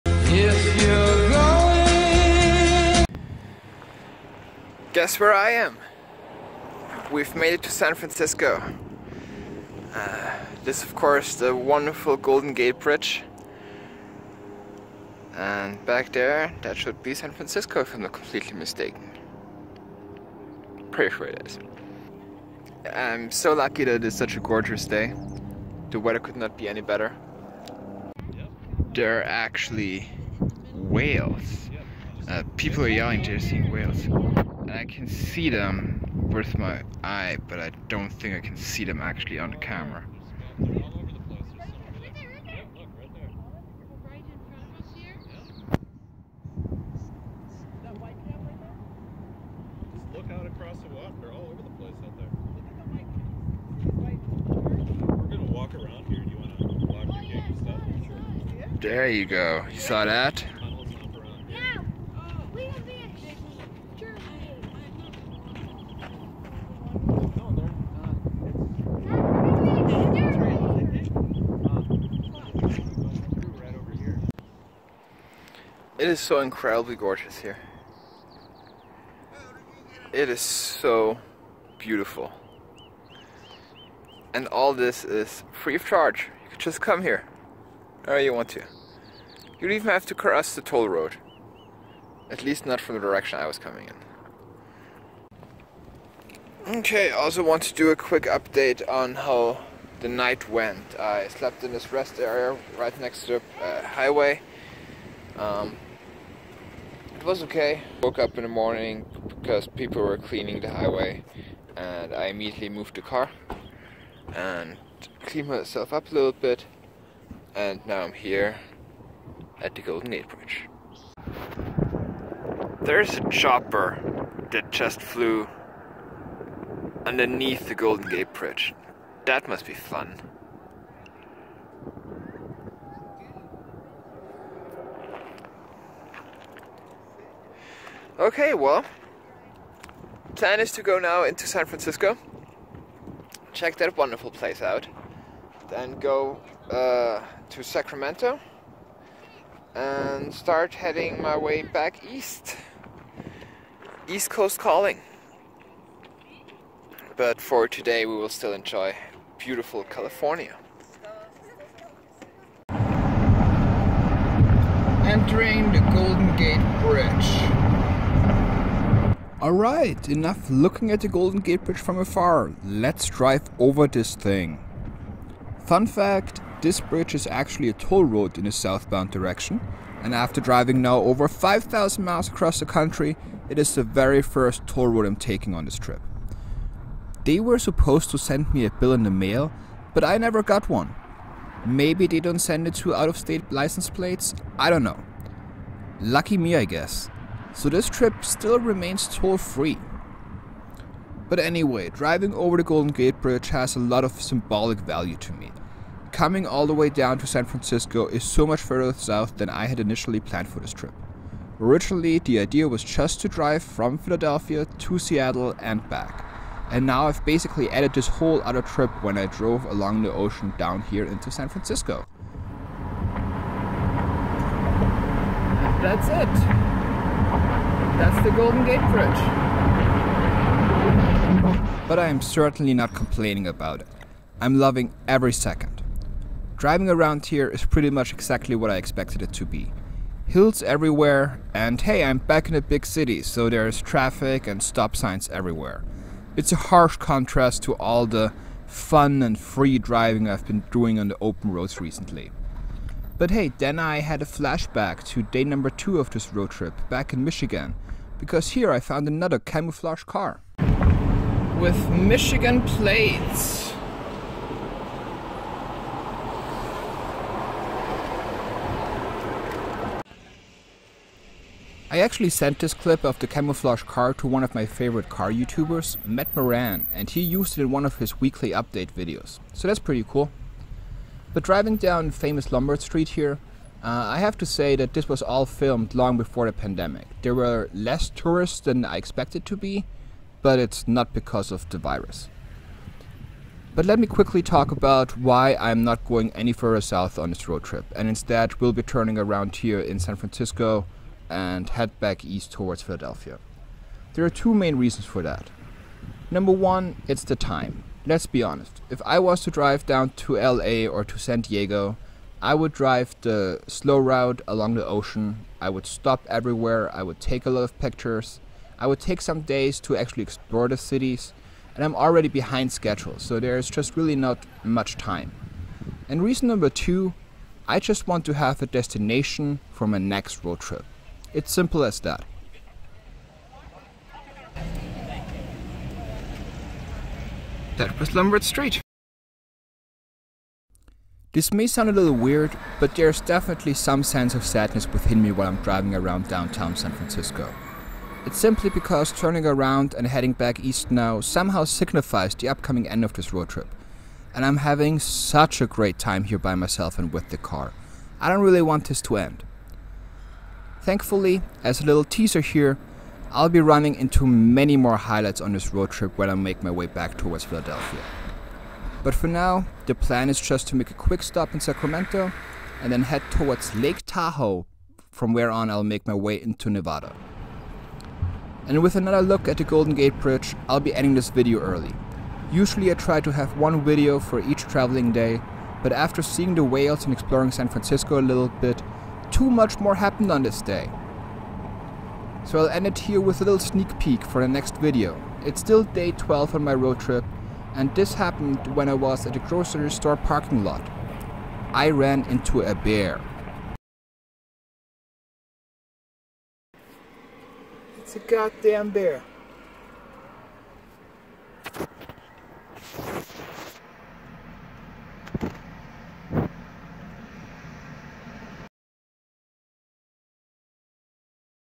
I Reach The Golden Gate Bridge | USA Camaro Road Trip Ep 12
Some surprise whale watching at the Golden Gate Bridge in San Francisco, California!

I just parked the Camaro for some pictures and next thing I know is people pointing out whales in the water - there were so many!

Shout out to @MattMaranMotoring whom I sent a video of a camouflaged car I came across. That's my second one on this road trip!

Also included a full drive through of Lombard Street

Travel Vlog